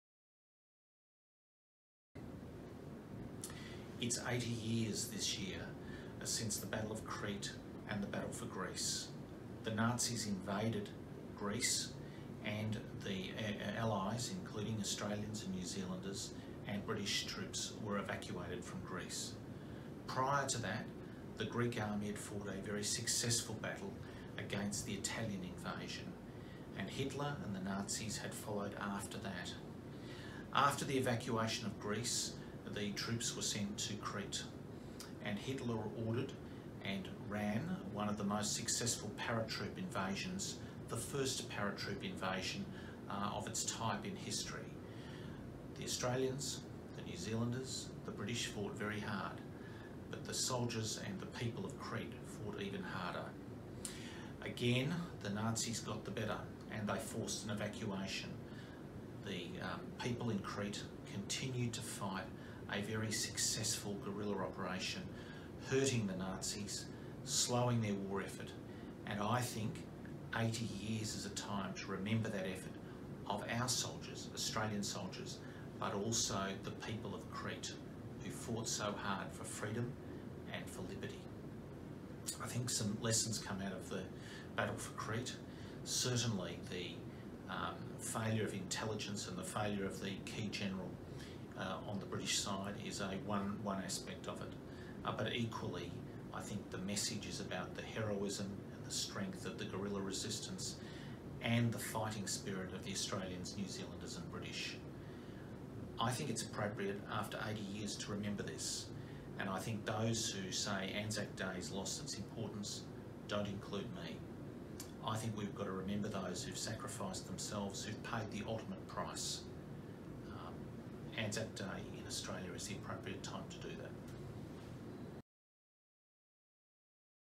David Davis MP talks about the commemoration of 80 years of the campaign in Greece & Crete
This year is also the commemoration of 80 years of the campaign in Greece and Crete.  After the Nazis invaded Greece, proud Anzacs fought a rear guard action and were evacuated to Crete.  The Nazis invaded Crete with the major battle in May 1941 – a paratroop invasion.  The brave resistance by Australian, New Zealand and British troops (noting their subsequent evacuation) was more than matched by the heroic guerrilla campaign fought by the people of Crete.